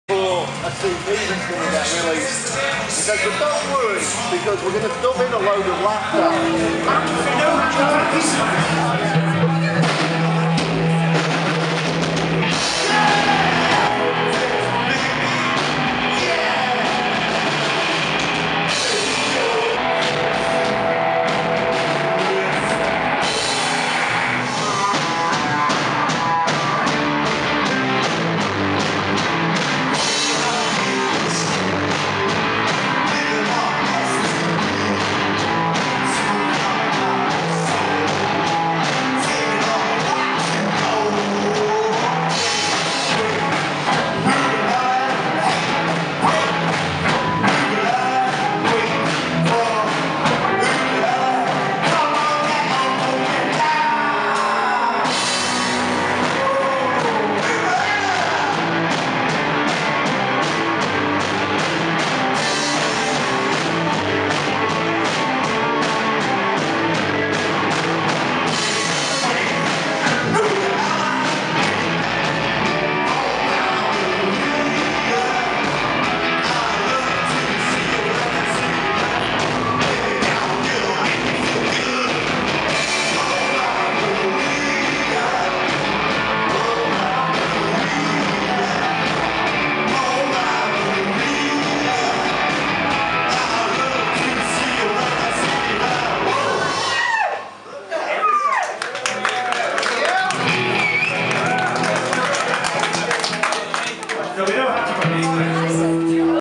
Star Spangled Banana @ EBGBs Liverpool 26.11.16
Star Spangled Banana do their bubblegrunge thing in EBGBs Liverpool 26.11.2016